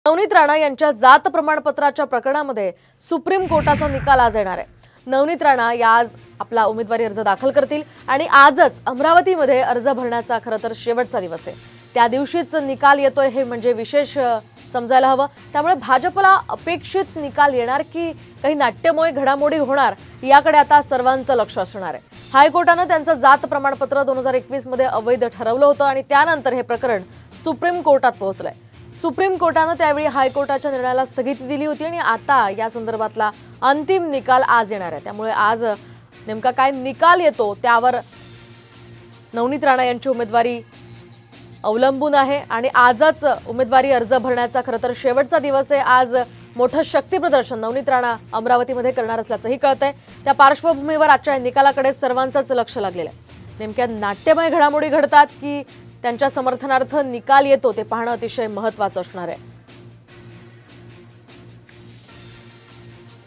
Navneet Rana News । नवनीत राणांचं जात प्रमाणपत्र प्रकरण, काय निकाल लागेल? | Marathi News
Navneet  Rana  News । नवनीत राणांचं  जात प्रमाणपत्र प्रकरण, काय निकाल लागेल? | Marathi News |SSG

नवनीत राणा यांच्या जात प्रमाणपत्राच्या प्रकरणाचा आज सुप्रीम कोर्टाकडून आज निकाल जाहीर होणार. दरम्यान नवनीत राणा उमेदवारी अर्ज सुद्धा आज दाखल करतील अशी माहिती समोर आली. भाजपाला अपेक्षित निकाल येईल की,  नाट्यमय घडामोडी होणार यांकडे सर्वांचे लक्ष असणार आहे. हायकोर्टानाई त्यांचं जात प्रमाणपत्र २०२१ मध्ये अवैध ठरवलं होतं  आणि ते प्रकरण सुप्रीम कोर्टात पोहचलं. आजच्या निकालावर नवनीत राणा यांची उमेदवारी ठरेल.